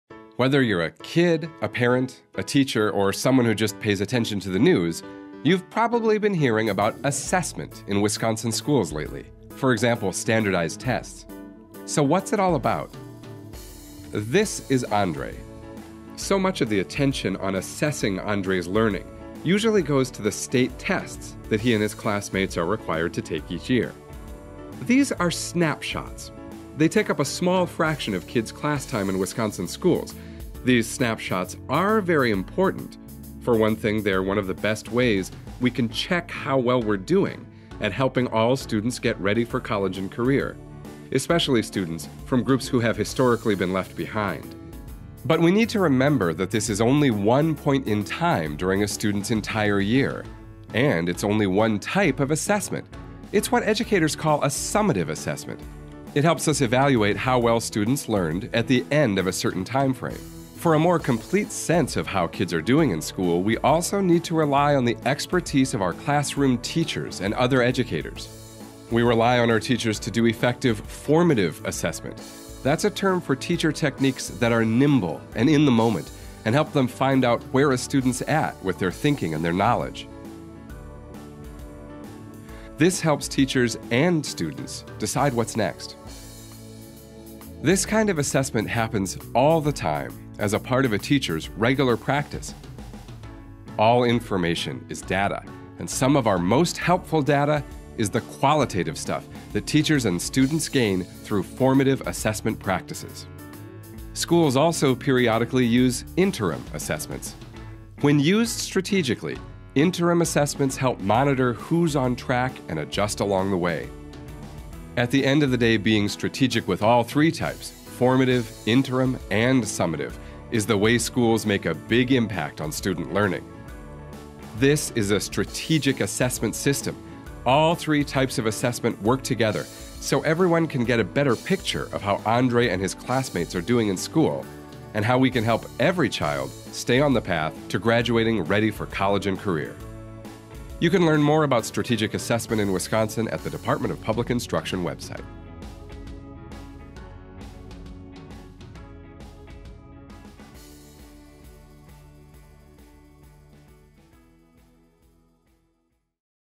What's Assessment All About?
How do we keep tabs on whether our students are on track to graduate ready for college and career?

This video gives an overview of what educational assessment is, and how different types of assessment should work together.